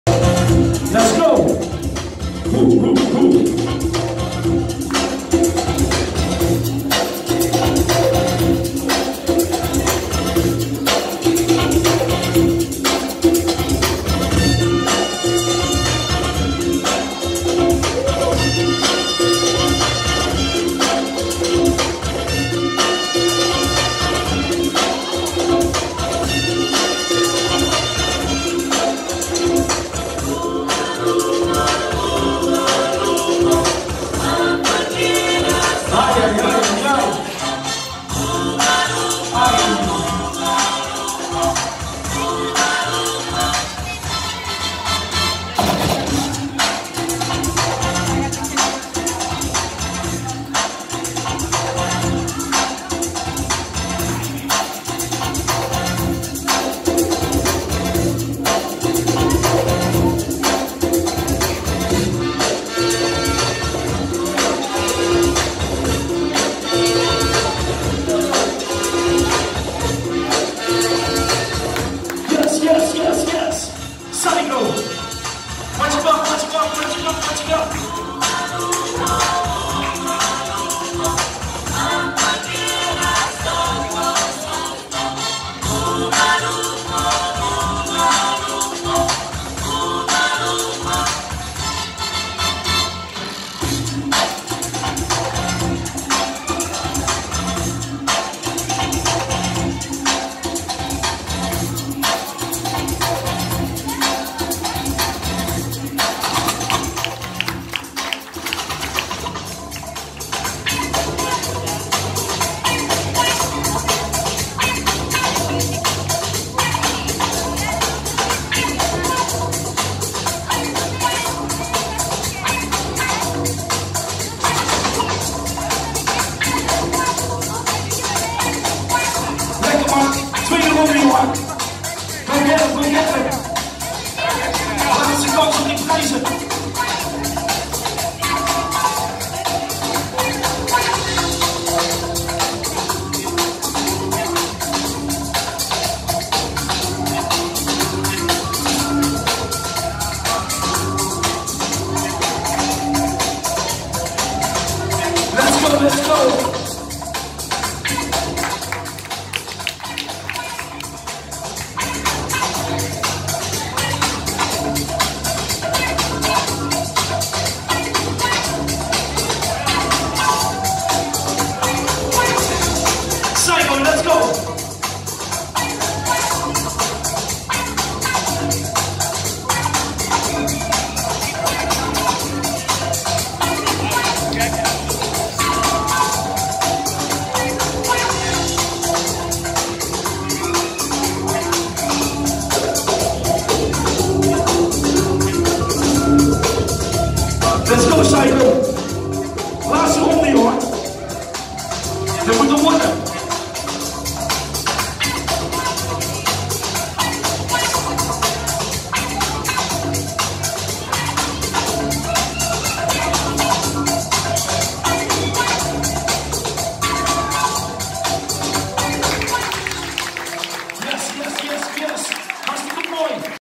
NBL 2019 | Den Bosch | Aiden vs Seigo
NBL Den Bosch! Zie hier de finale terug tussen Aiden en Seigo.

Winner: Seigo
Runner Up: Aiden